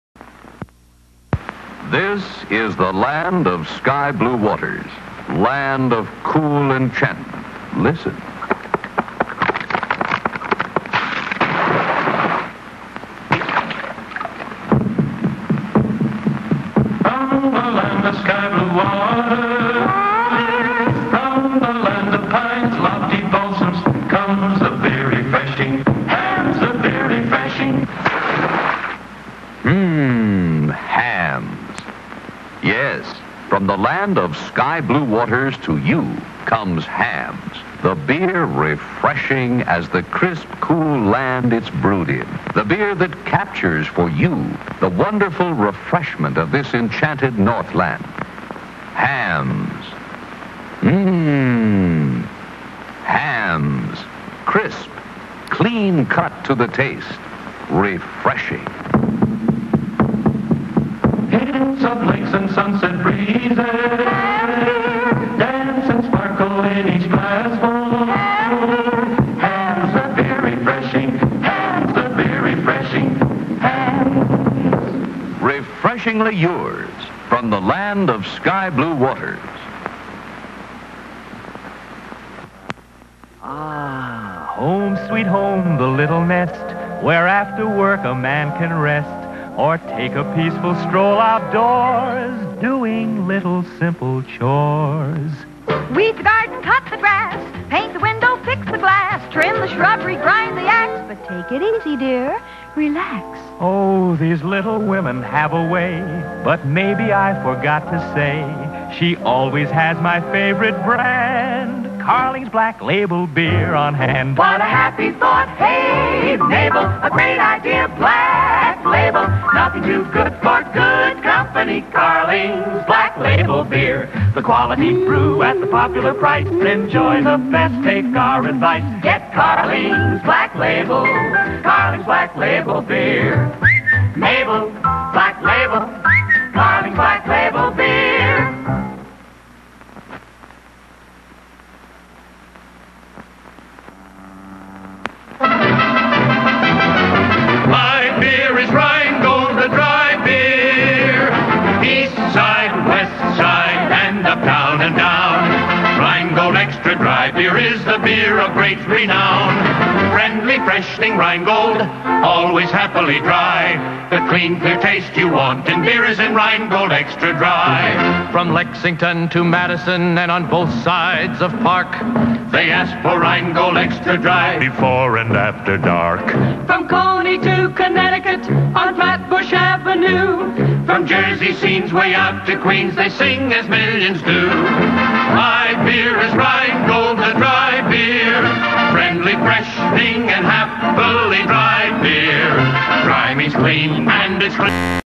Vintage Beer Commerical
3 US beer commercials from (I think) the 60's. I found in the Internet Archive